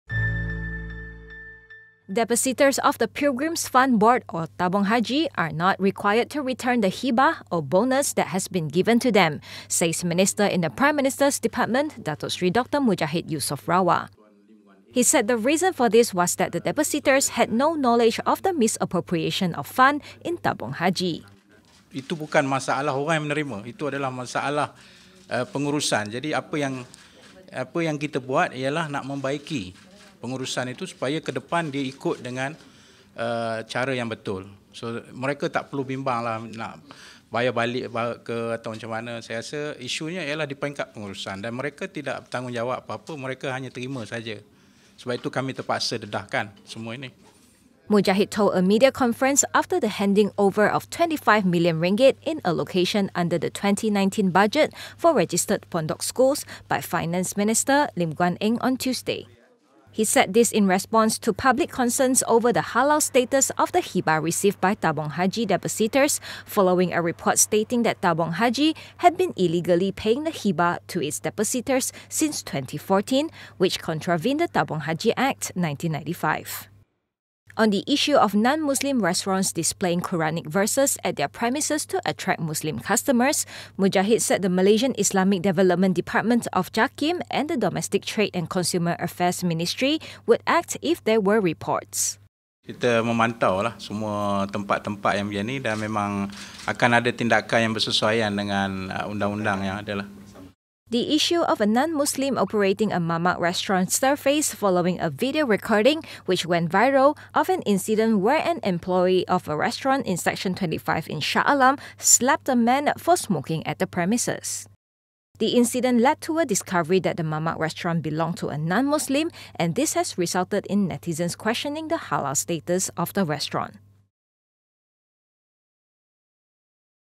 Mujahid: Tabung Haji depositors need not return 'hibah'
Depositors of the Pilgrims Fund Board or Tabung Haji are not required to return the "hibah" (bonus) that has been given to them, says Minister in the Prime Minister's Department Datuk Seri Dr Mujahid Yusof Rawa.

He said the reason for this was that the depositors had no knowledge of the misappropriation of fund in Tabung Haji.

 This followed a report which stated that Tabung Haji had been illegally paying the "hibah" to its depositors since 2014, which contravened the Tabung Haji Act 1995 since it had more debts than assets in the previous years.